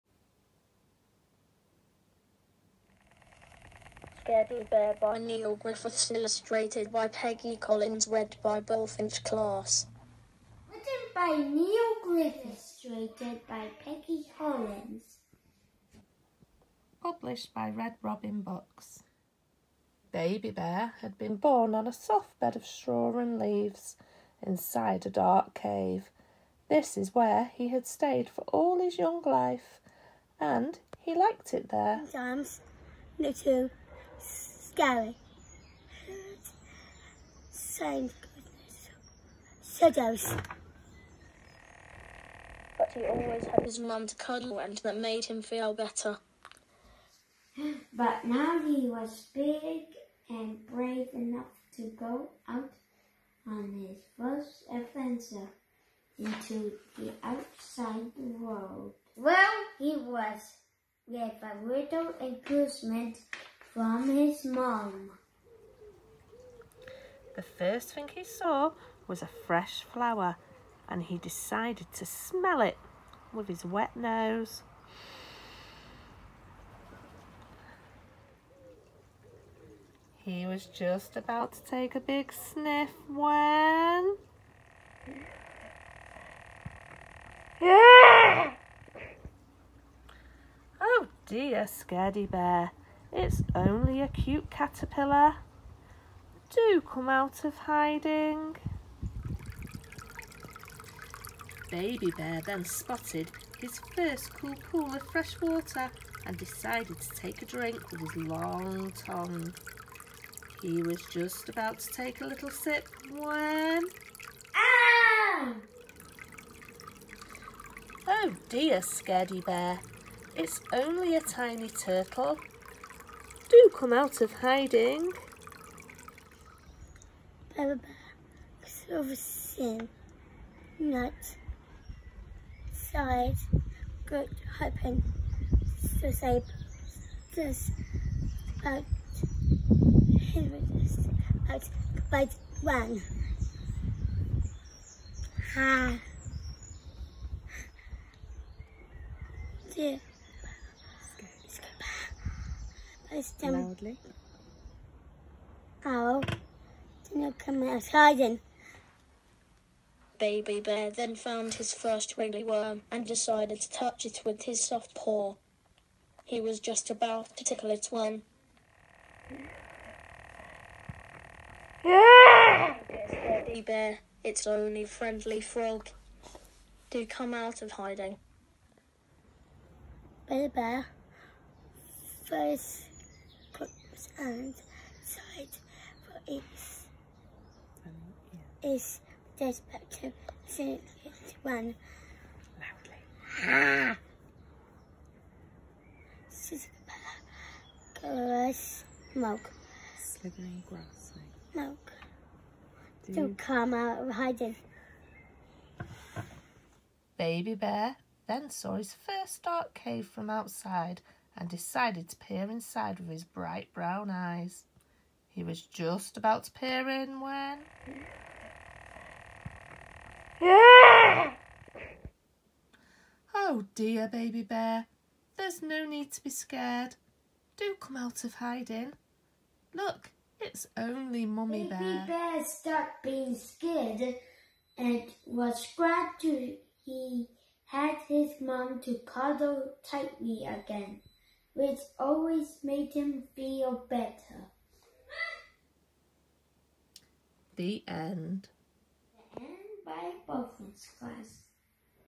Scaredy Bear Bullfinch Class
The story of Scaredy Bear brilliantly told by Bullfinch Class, from St Nicholas Special School in Chippenham. Our thanks to their amazing teacher Paula Rutherford for allowing us to show this to the world! You are all Brilliant!